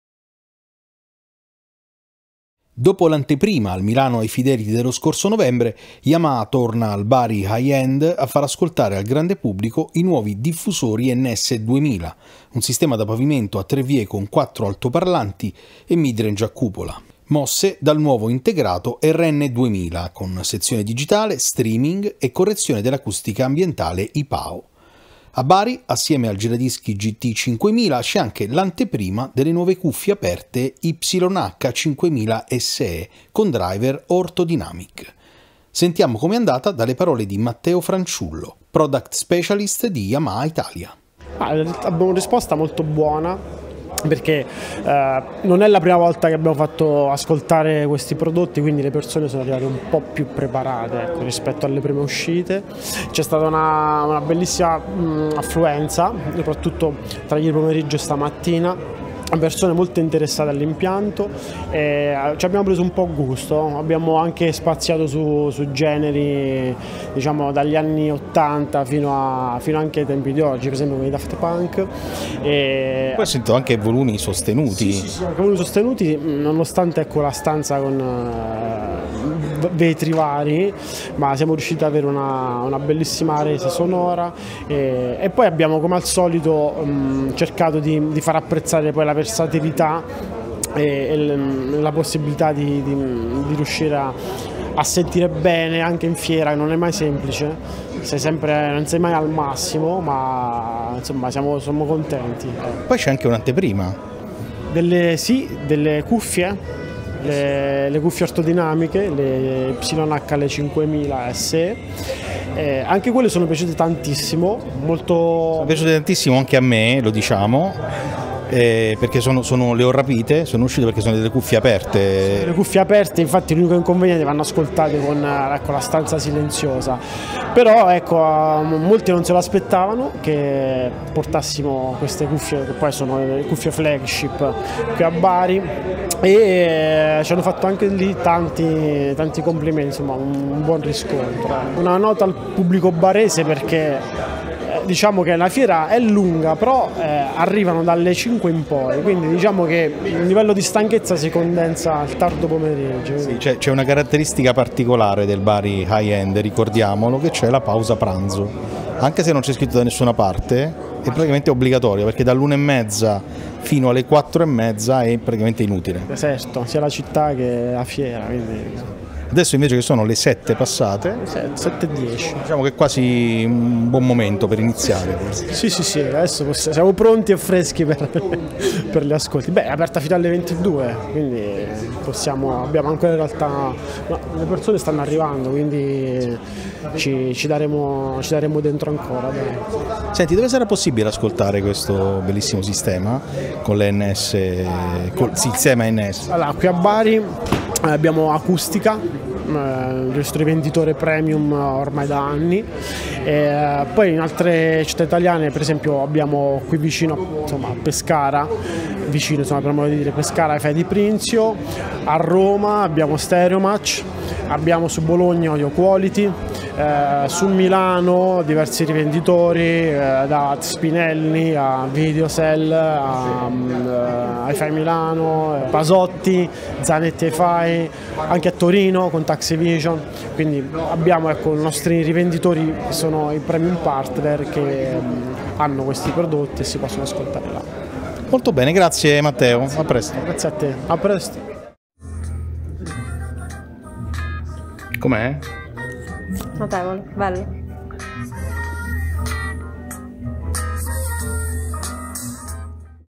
Yamaha NS-2000, R-N2000, GT-5000 e YH-5000ES al Bari Hi-End 2023
Yamaha torna al Bari Hi End con i diffusori da pavimento NS2000, integrato RN2000A e giradischi GT-5000 e con l'anteprima delle nuove cuffie aperte YH-5000SE con driver driver ORTHODYNAMIC...